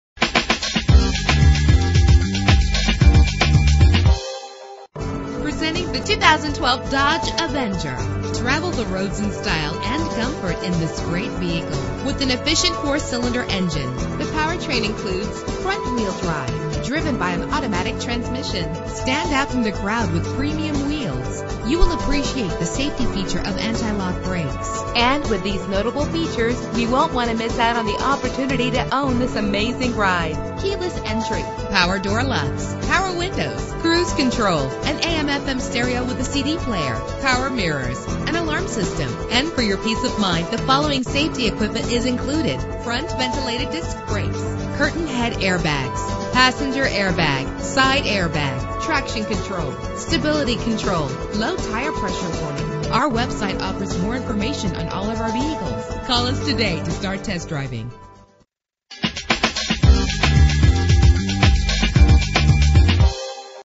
2012 Dodge Avenger - Carlsbad CA
Year: 2012
Make: Dodge
Model: Avenger
Trim: SE
Engine: 2.4 liter inline 4 cylinder 16 valve
Transmission: 4-Speed Automatic
Color: Black
Mileage: 20371
Address:
5555 Car Country Dr